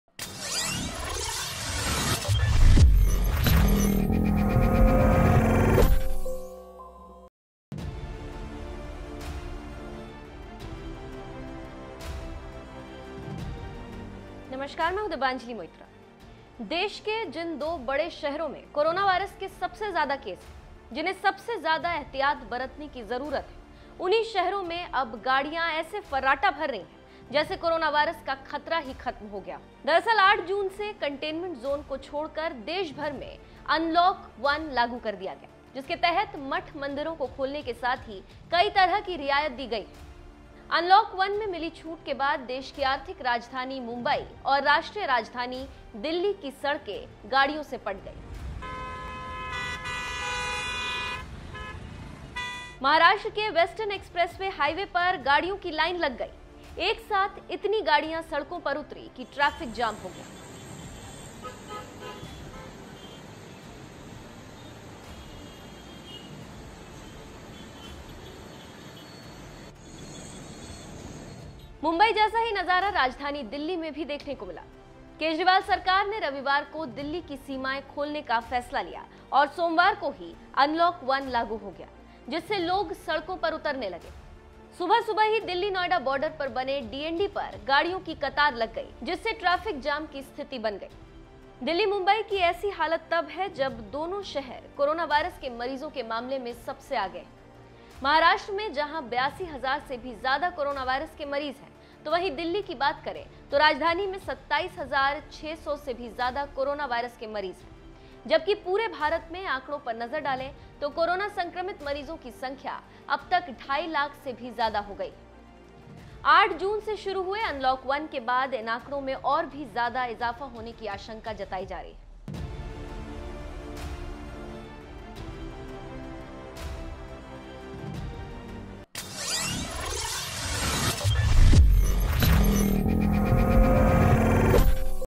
Unlock 1: सबसे ज्यादा Corona Case वाले Delhi और Mumbai का हाल देखिए !
Unlock 1 शुरू होते ही Mumbai, Delhi की सड़कों पर लग गई गाड़ियों की लाइन !